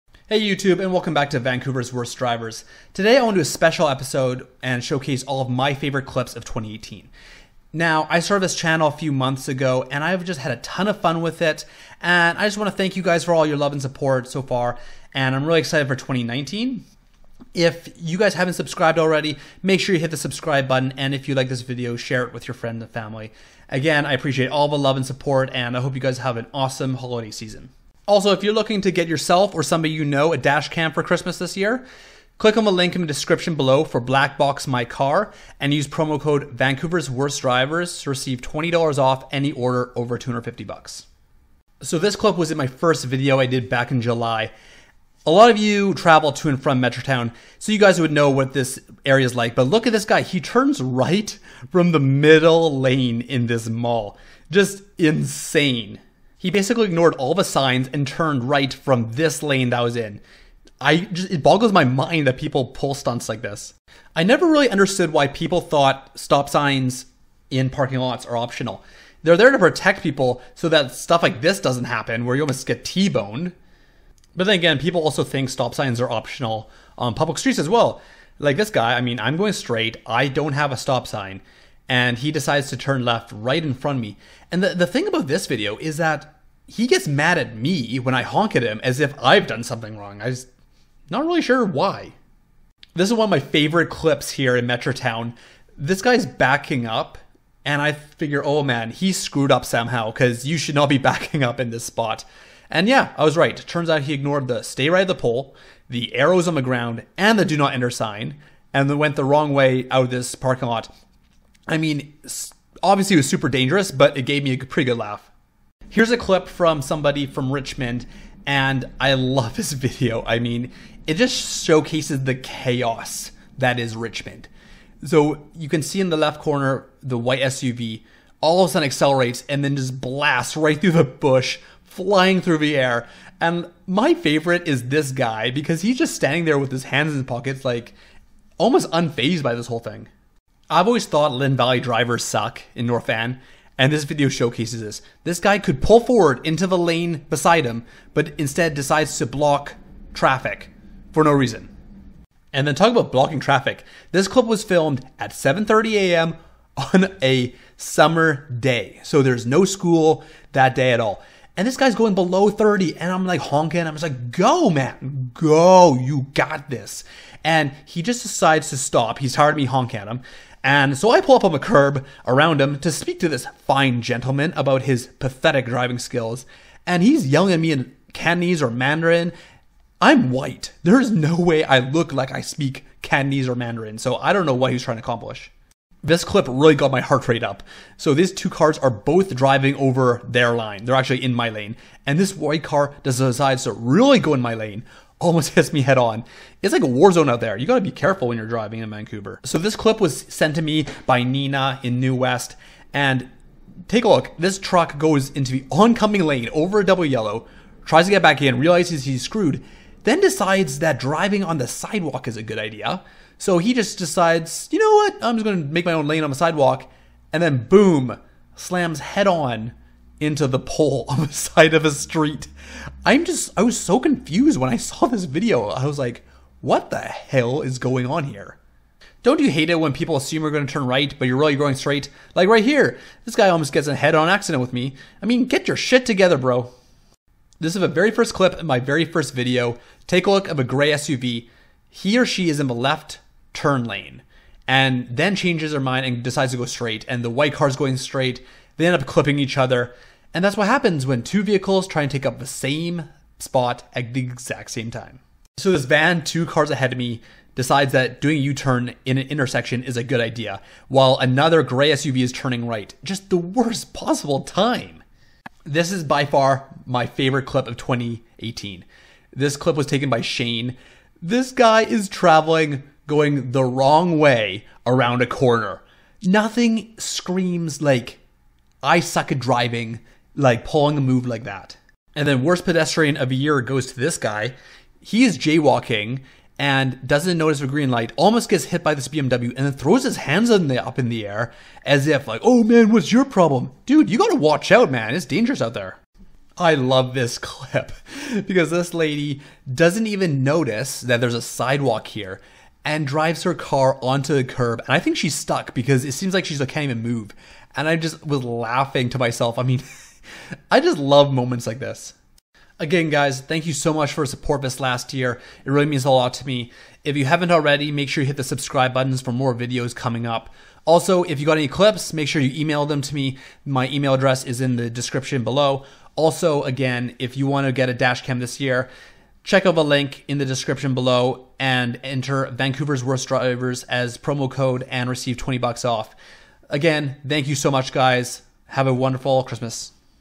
Best Dashcam Videos of 2018 | Vancouver's Worst Drivers
Here are my favorite clips from 2018! If you have any clips you want to send me for my next video email them to me and I'll include them in my next video. Hope you all have a wonderful Christmas and stay safe on the roads!